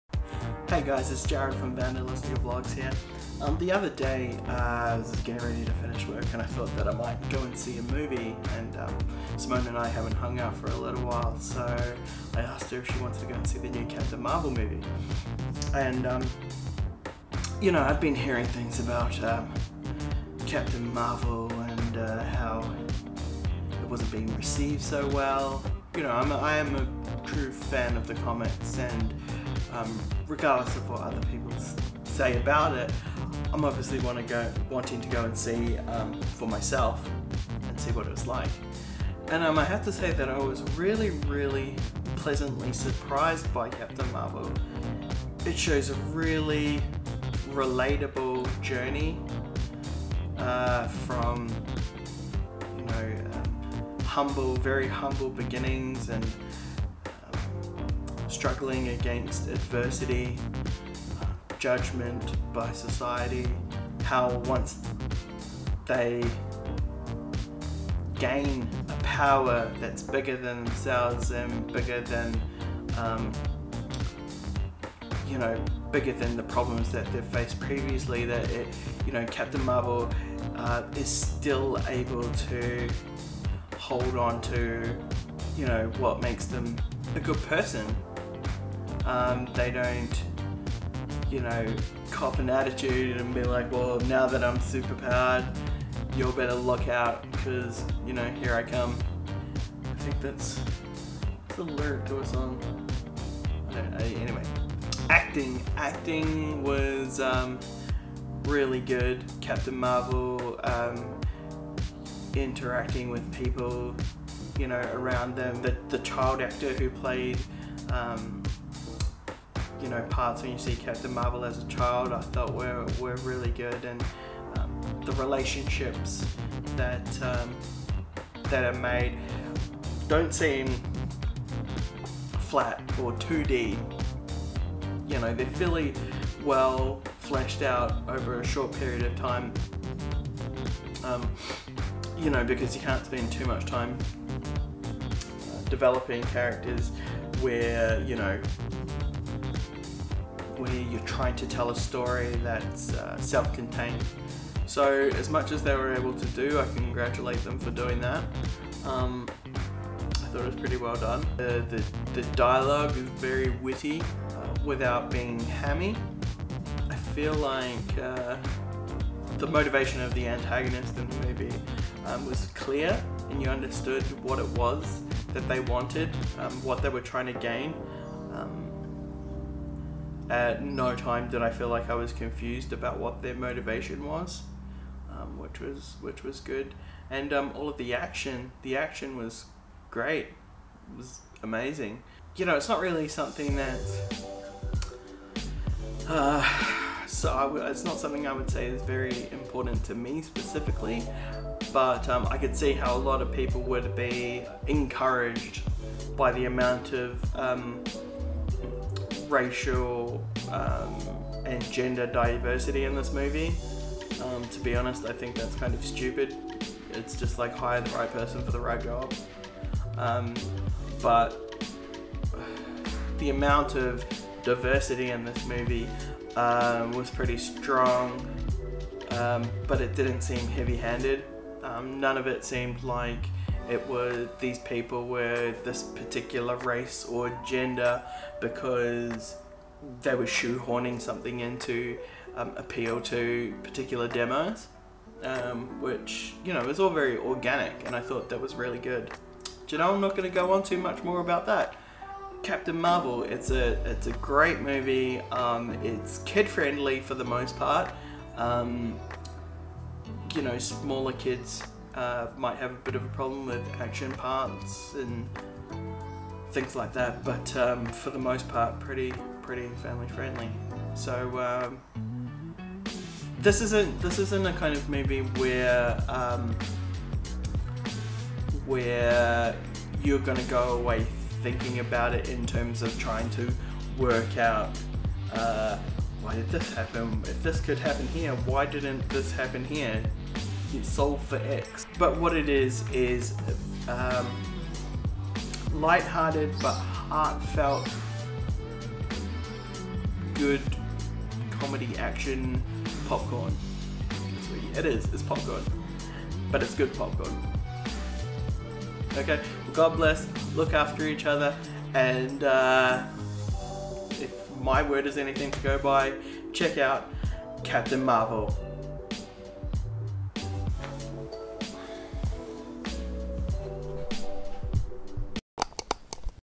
CAPTAIN MARVEL: MY IMPRESSIONS
Simone and I went to see the Captain Marvel movie the other day and we both really enjoyed it!  I thought I'd let you know what I thought of it.